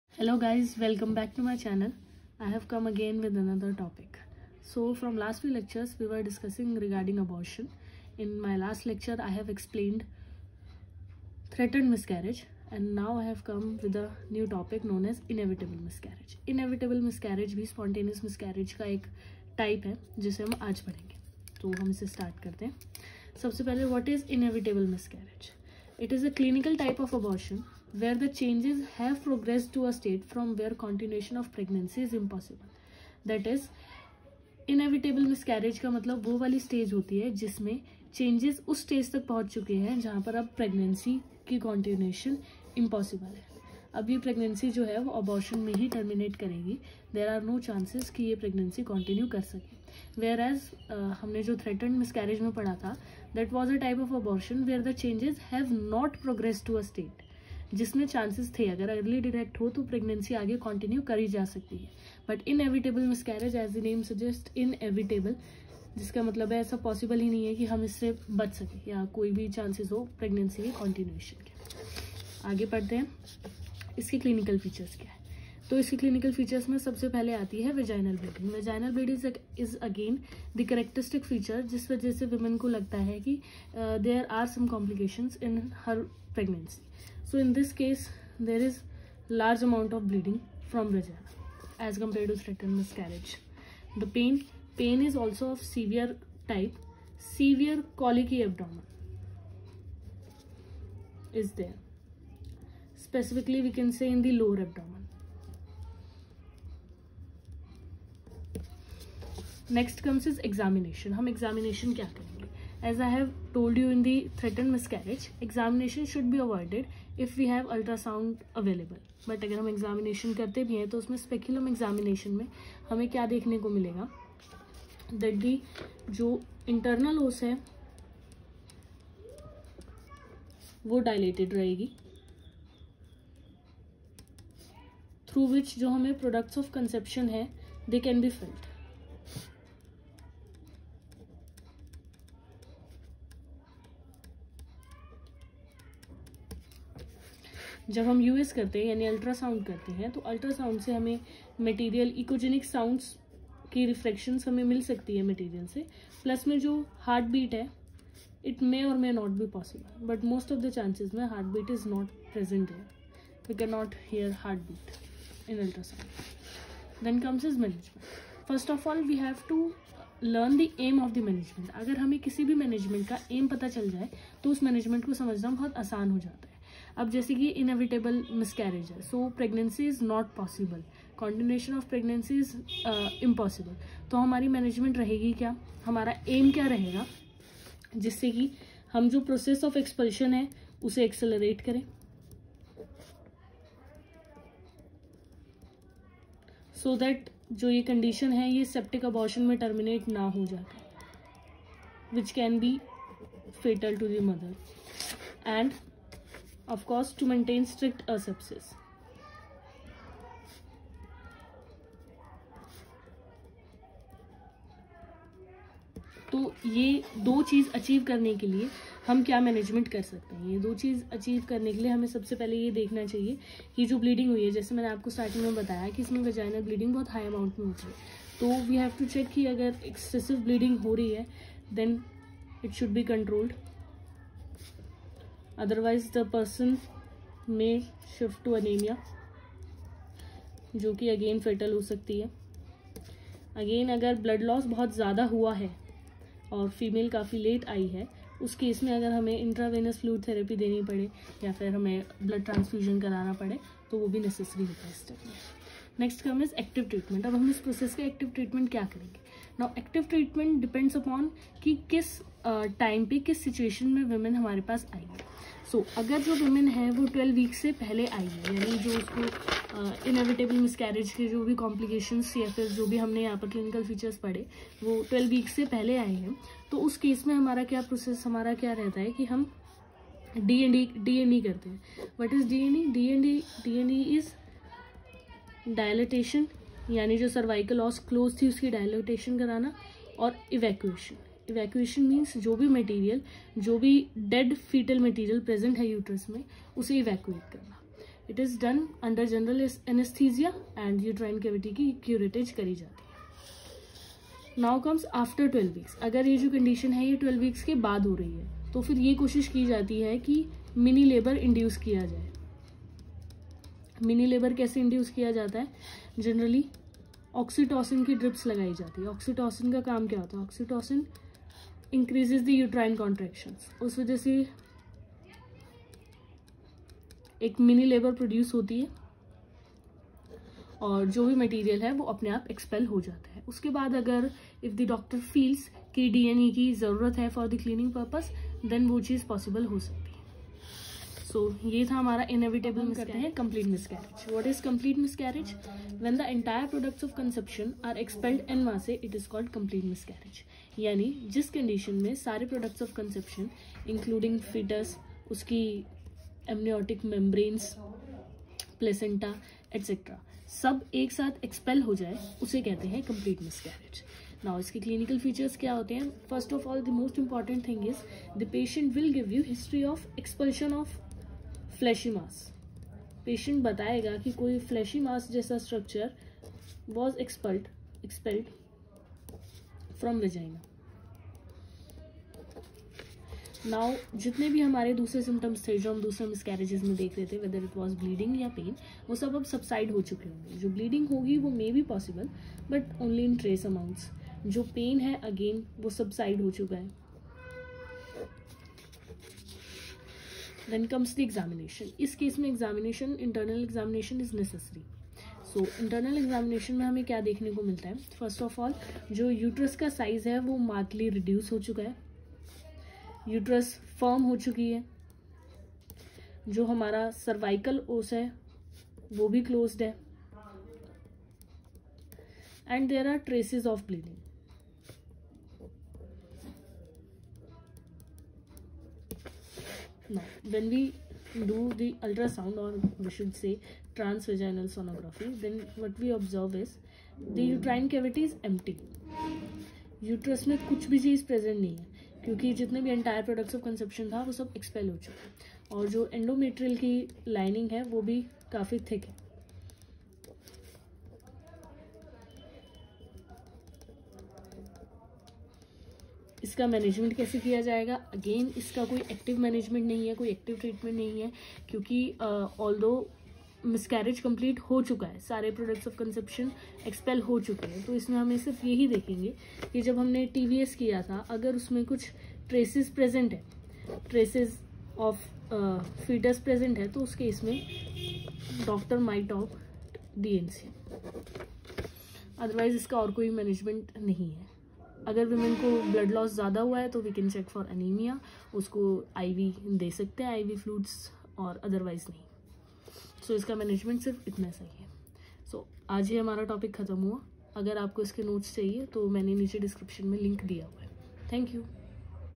Complete miscarriage lecture in hindi| Inevitable miscarriage| Hemorrhage in early pregnancy
This video explains complete miscarriage and inevitable miscarriage in detail which can be a reason for hemorrhage in early pregnancy.